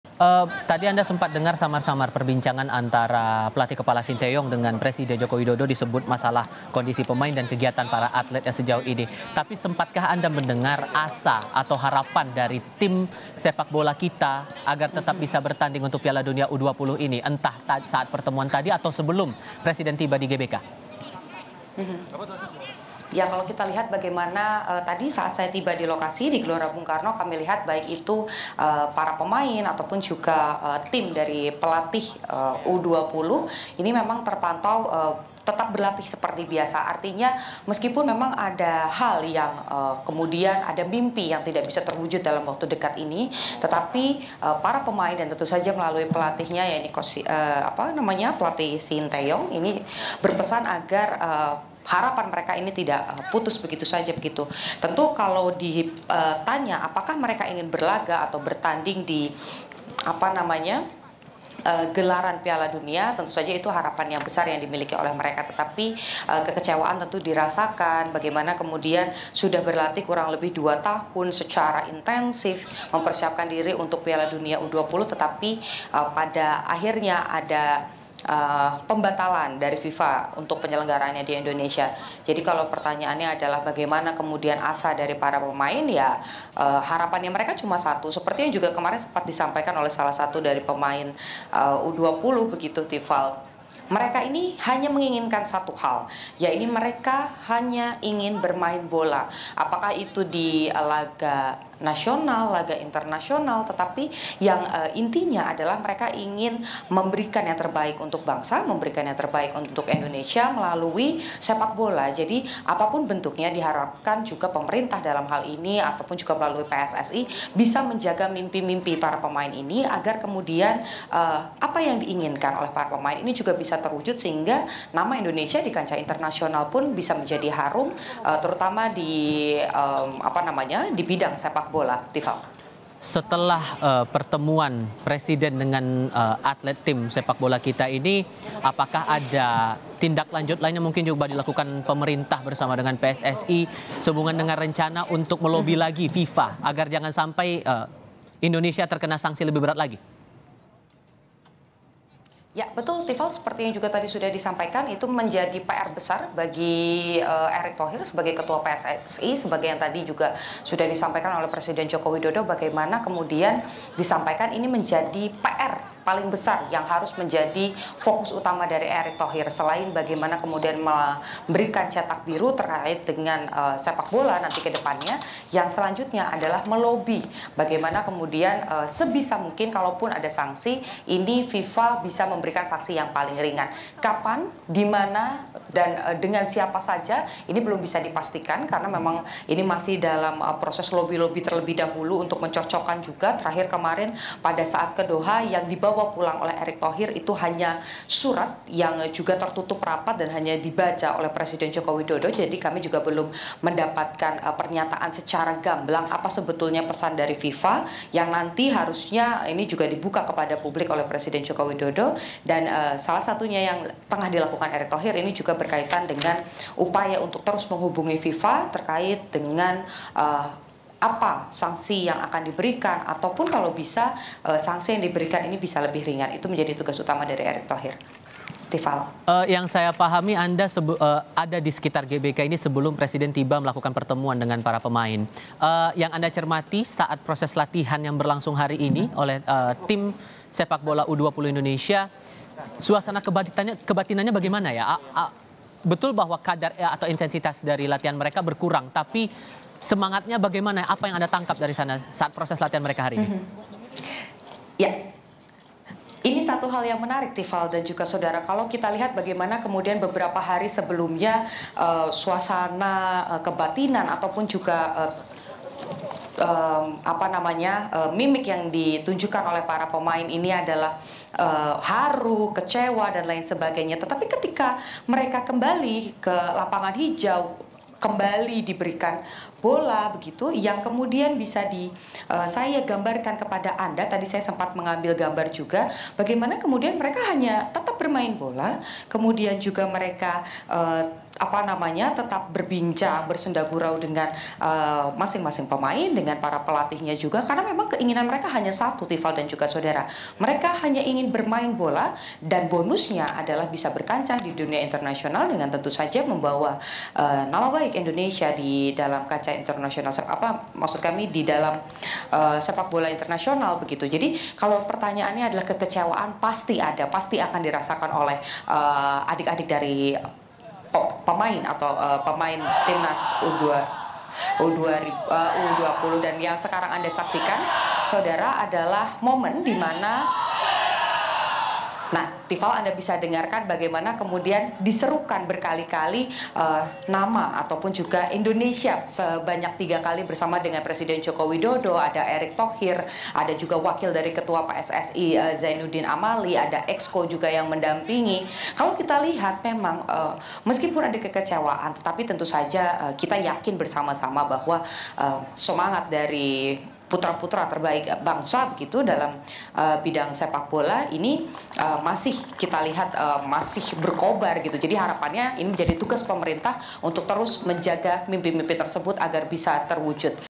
Shin Tae Yong Sedih dan Kecewa: Timnas Sudah Berlatih Bertahun-tahun untuk Piala Dunia
JAKARTA, KOMPAS.TV - Presiden Joko Widodo menemui langsung Tim Indonesia U-22 dan U-20 yang batal bertanding menyusul pembatalan Indonesia sebagai tuan rumah Piala Dunia U-20.

Jokowi bertemu para pemain timnas di Stadion GBK Senayan, Jakarta, Jumat (1/4/2023).

Ini merupakan pertama kalinya Presiden Jokowi menemui Pelatih Shin Tae Yong usai pembatalan helatan Piala Dunia U-20 di Indonesia.

Terakhir kali Shin Tae Yong menyebutkan kekecewaannya dan kesedihannya bahwa Timnas U-20 Indonesia tidak bisa bermain di Piala Dunia U-20, padahal Timnas U-20 sudah jalani pelatihan selama 3 tahun 6 bulan demi mempersiapkan pertandingan di helatan Piala Dunia U-20.

Selain mendapat keterangan mengenai perkembangan tim dari Pelatih Shin Tae Yong, Jokowi juga mendengarkan curhatan Tim Garuda Muda yang sempat kecewa dengan pembatalan Piala Dunia U-20 di Indonesia.

Baca Juga Jokowi Perintah PSSI Siapkan Blueprint Transformasi Sepak Bola untuk FIFA di